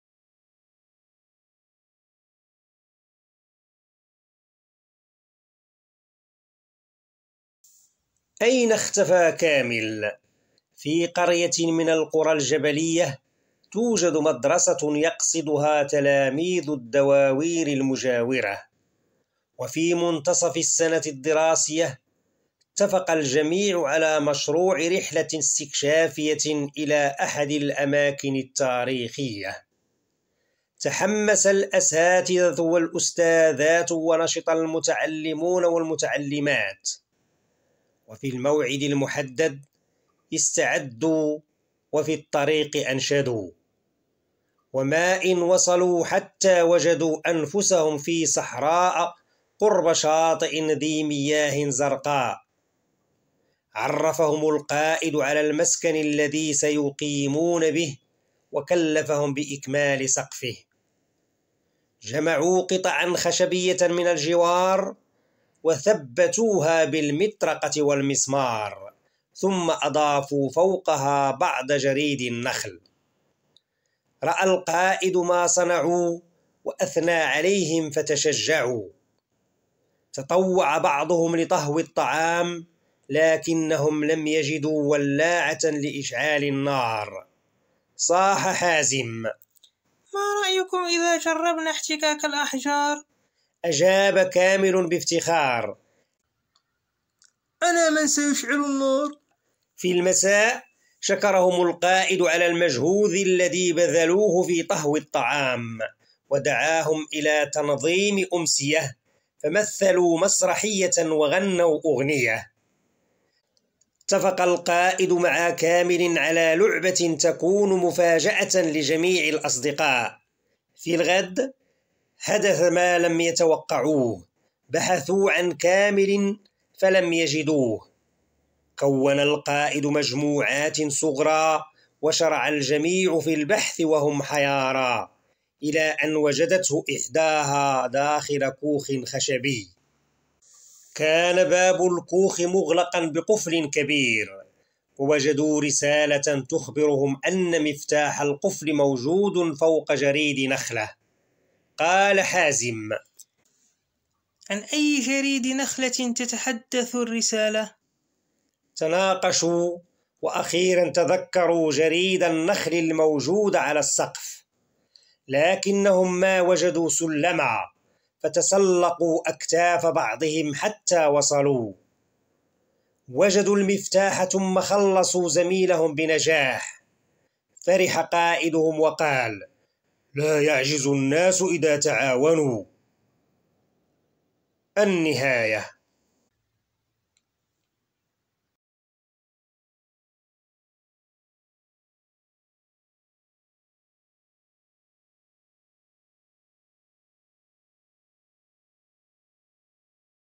أين اختفى كامل ؟
الحكاية 3 : أين اختفى كامل ؟ - مرشدي - المستوى الثالث
مرشدي في اللغة العربية المستوى الثالث ابتدائي

 🛑🛑    تجدون أيضا على نفس القناة الكتب و المستويات التالية    🛑🛑